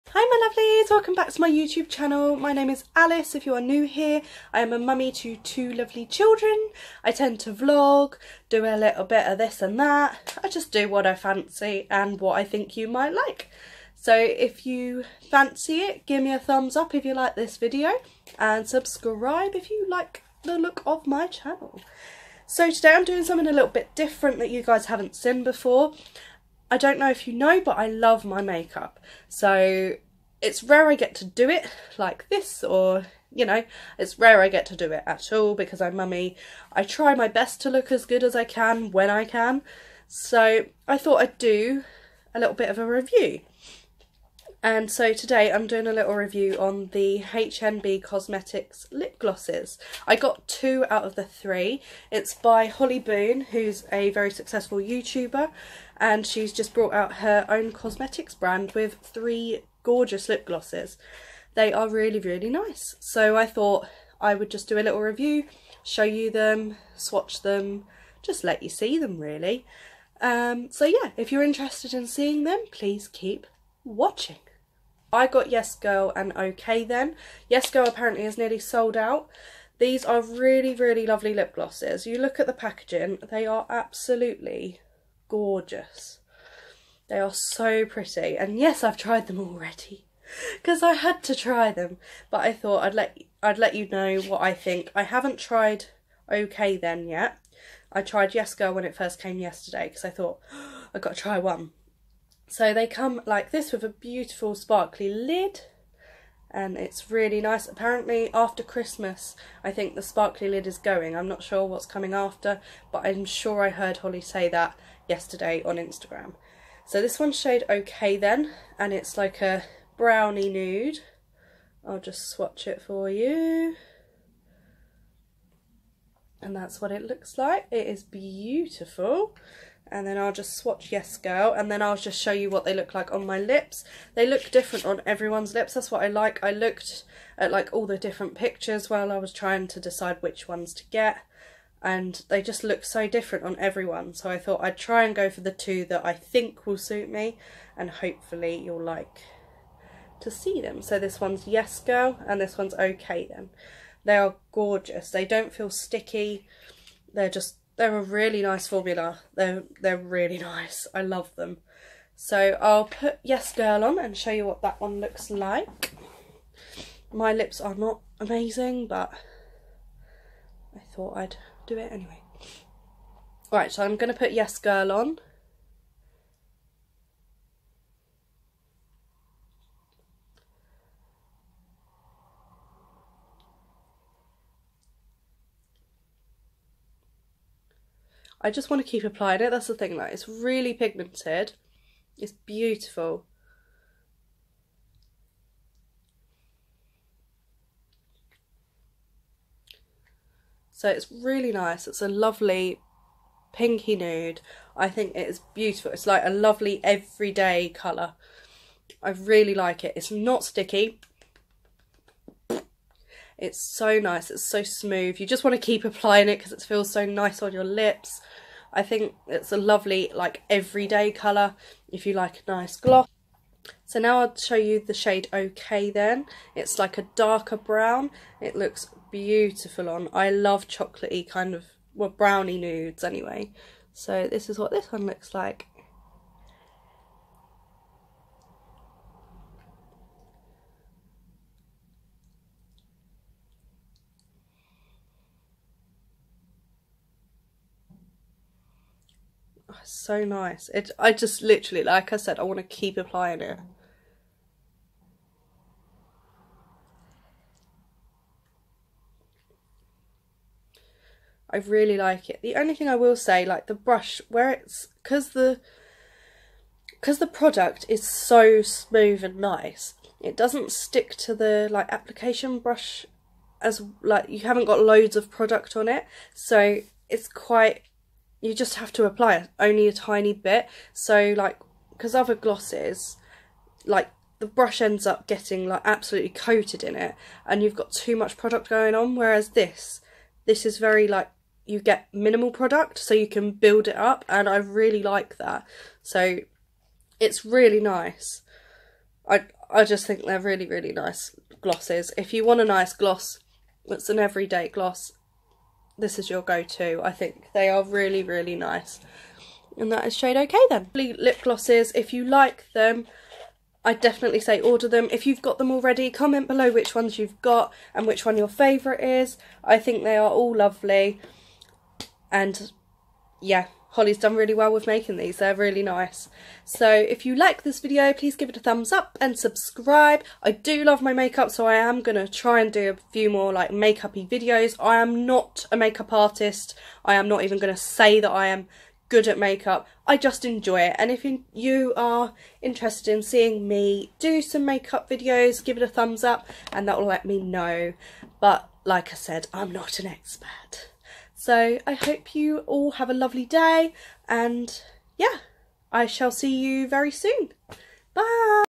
HNB COSMETICS REVIEW | HOLLY BOON LIP GLOSSES
So I decided to get some of these lipglosses as they've recently been released and I thought I'd review them and show you what they are like !